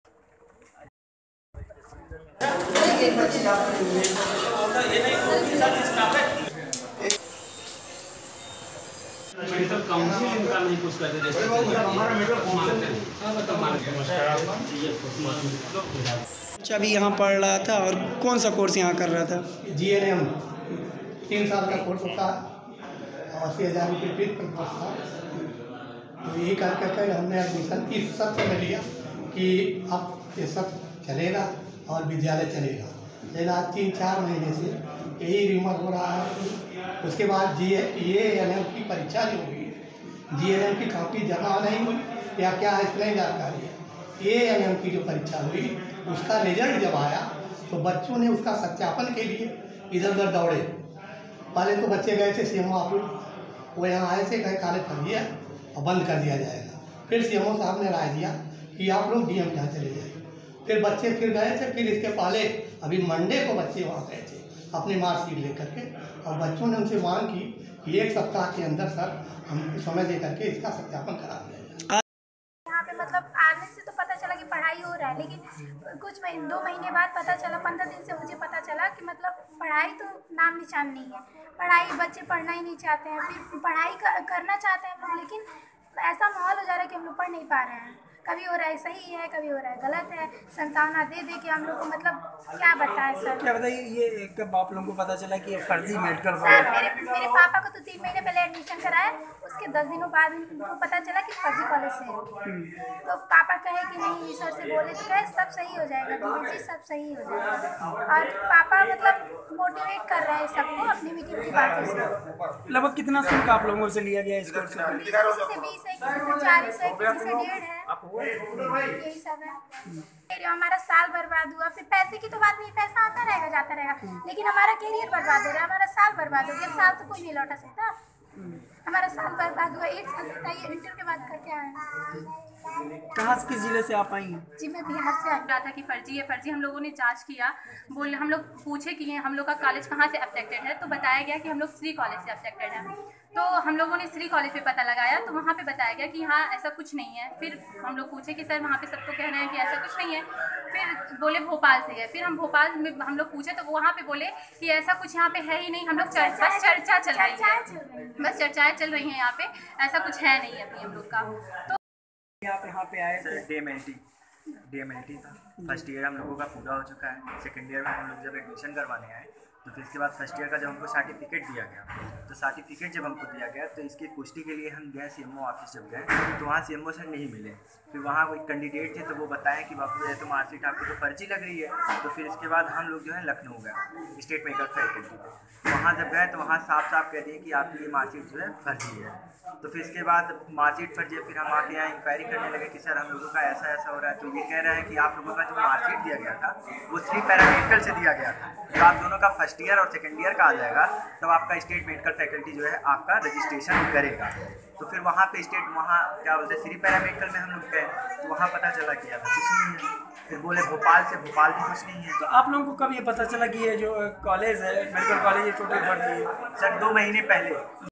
खबर का असर : बस्ती में फर्जी मेडिकल कॉलेज पर लगा ताला
बस्ती: जिले के मां वैष्णो पैरामेडिकल इंस्टिट्यूट में फर्जीवाड़े का खुलासा हुआ है. डीएम के निर्देश पर जांच अधिकारी ने एसडीएम और सीओ सदर की मौजूदगी में इस कॉलेज को सील कर दिया. यहां कार्रवाई के दौरान दो लोगों को पुलिस ने हिरासत में लिया है. वहीं कॉलेज प्रबंधक संग्राम और आरसी वर्मा फरार हो गए.